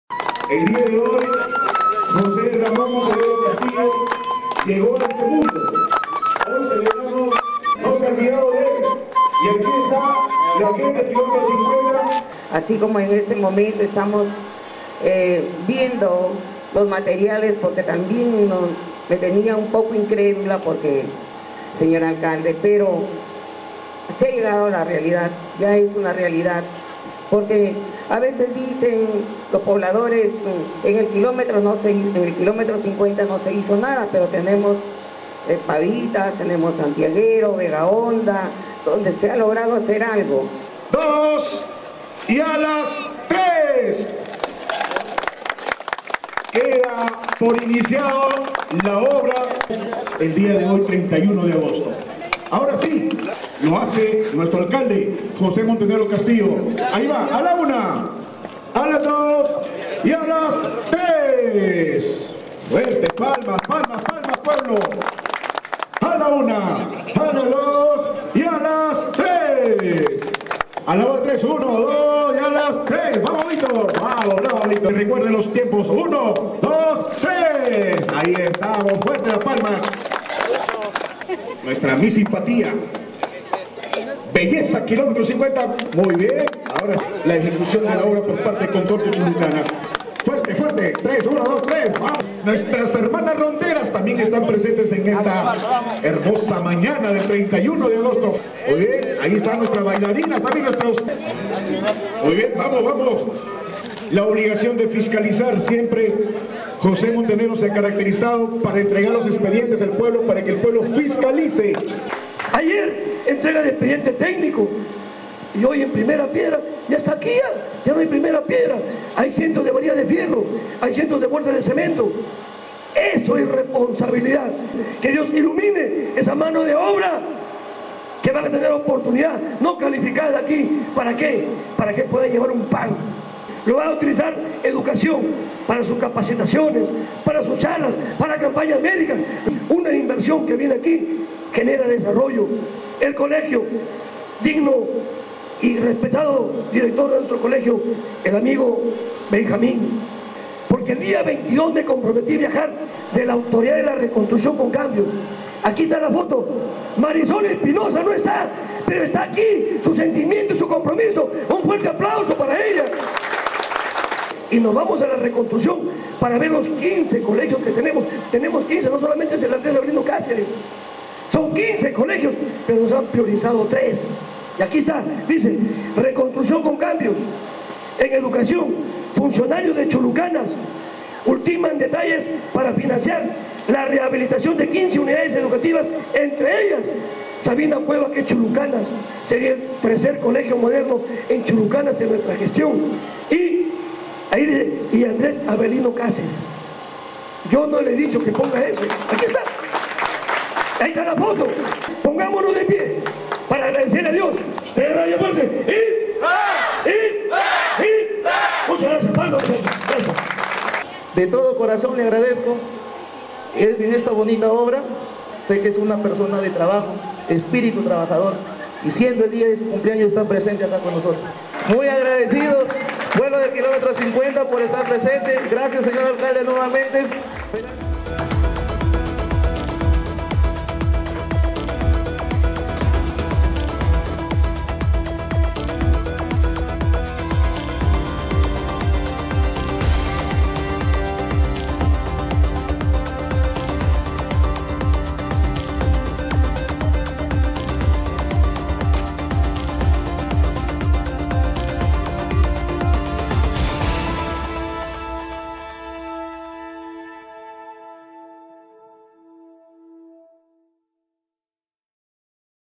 Construcción de Moderno Local Comunal en el Km. 50
Gracias a Buena Gestión de José Montenegro alcalde de Chulucanas se iniciaron los trabajos de la construcción del Moderno Local Comunal del Km. 50.